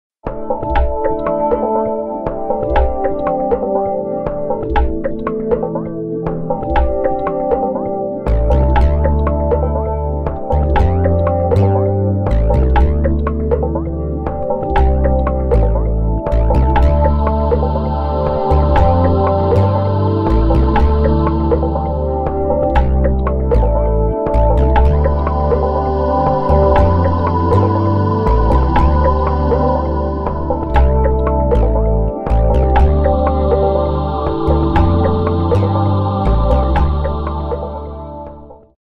Geminids 2011
Some Geminids captured by SK-1004X video module.